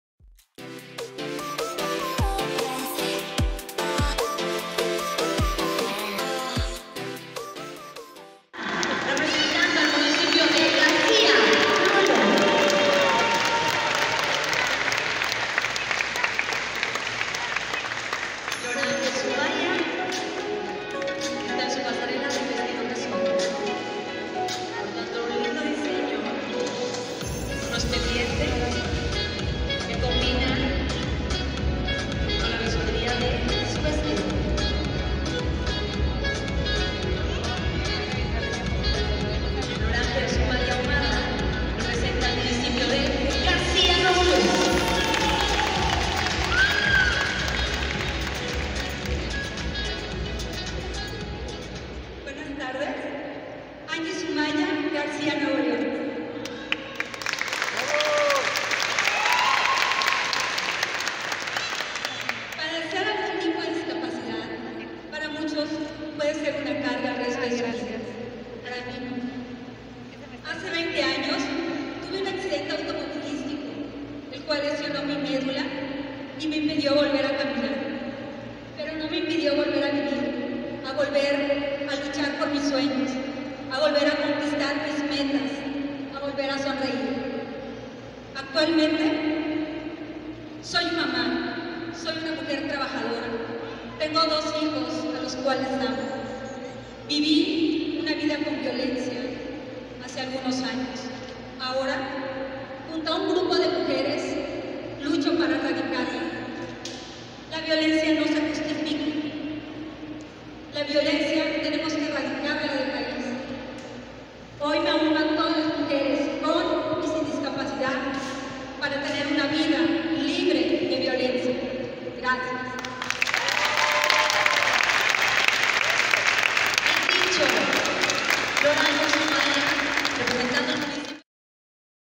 ANGIE ANGIE
El domingo 28 de fui invitado a un evento, Certamen de Belleza Miss WheelChair Nuevo León 2022, en San Pedro Garza García.

Y claro con gusto por ahí andaba, aun y que desconozco este tema pero quiero felicitar a la personas que organizaron y más por darle su lugar a personas con alguna discapacidad.

Le agradezco a Angie Zumaya por invitarme.
Y excelente su participación y por representar al Municipio de García.
 En este evento.